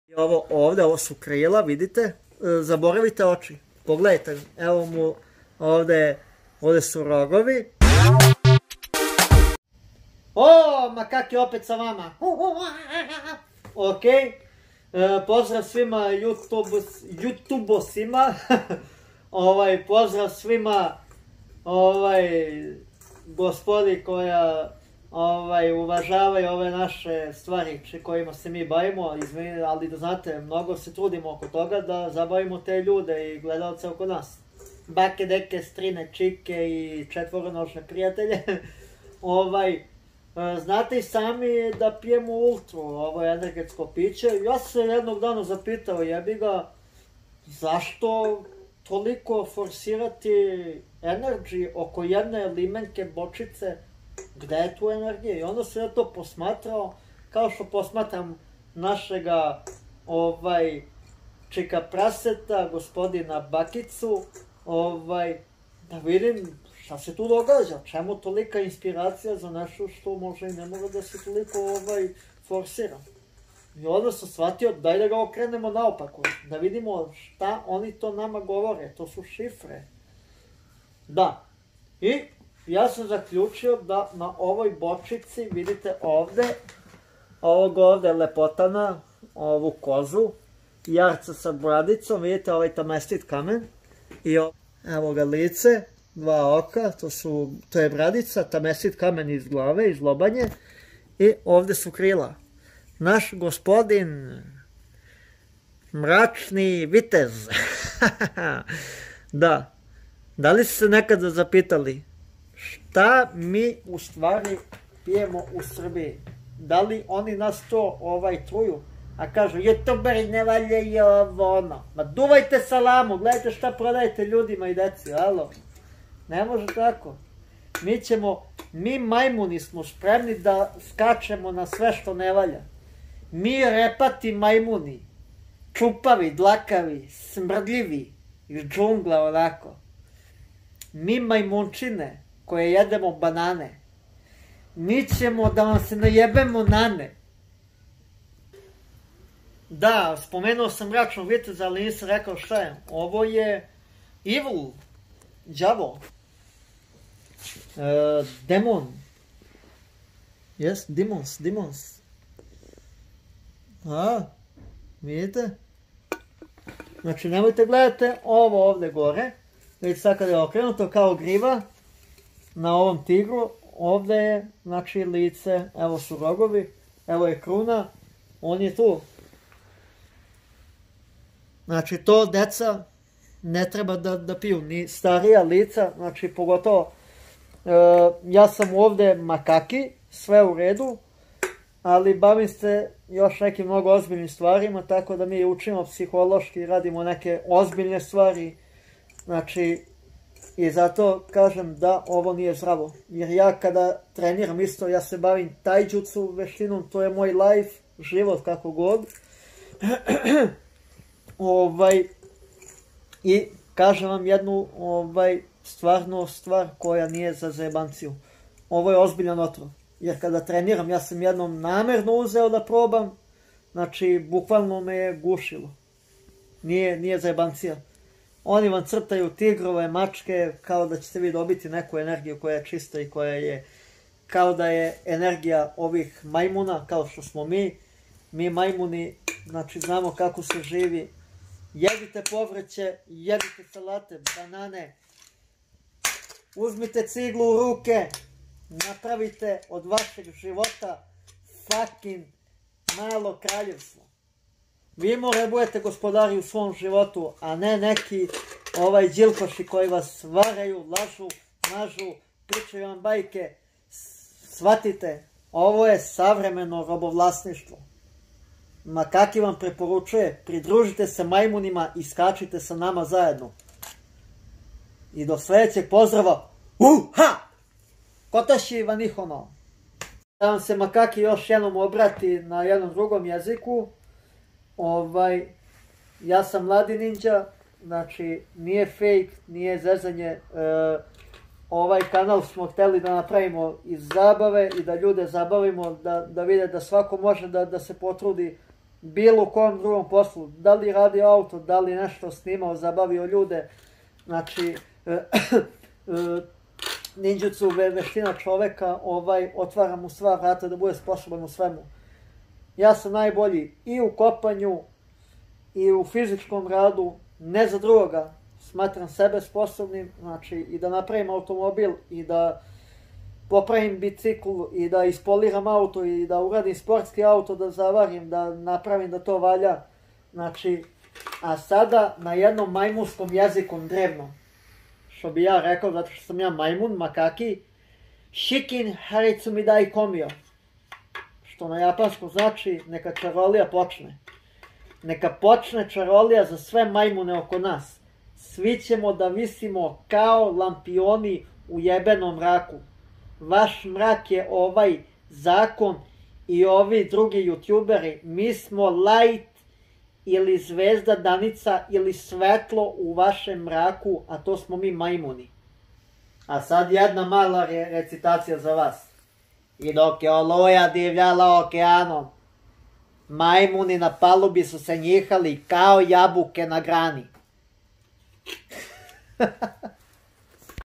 10 stvari koje niste zali o energetskim picima (mracna tajna)
Energetska pica red bull, guarana, ultra i tako dalje kriju u sebi mracnu tajnu mracnog viteza. Lepo uvezenu tajnu upakovanu u sarenu bocicu koja te mami kao mamac. A da li je sve tako sjajno i bajno doktor Makaki otkriva!?!